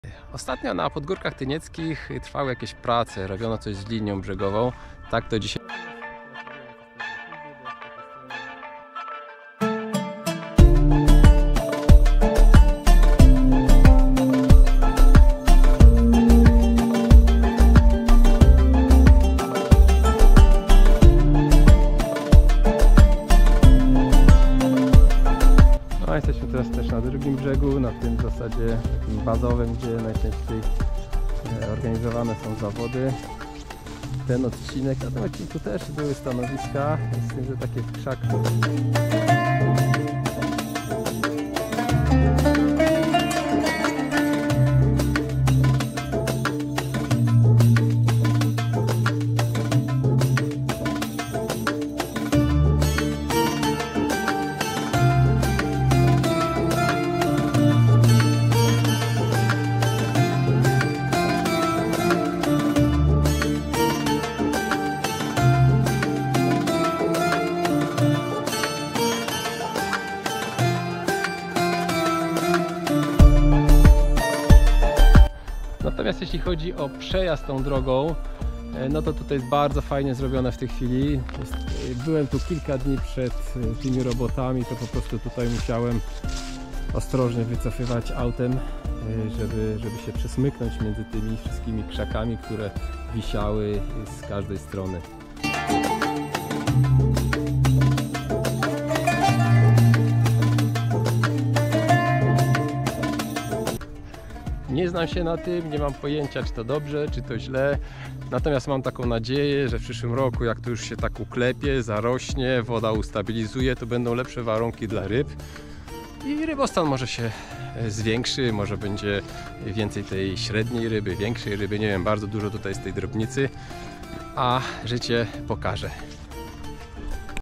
Pogórki - zryty brzeg!
Prace na Podgórkach - czy to dobry pomysł?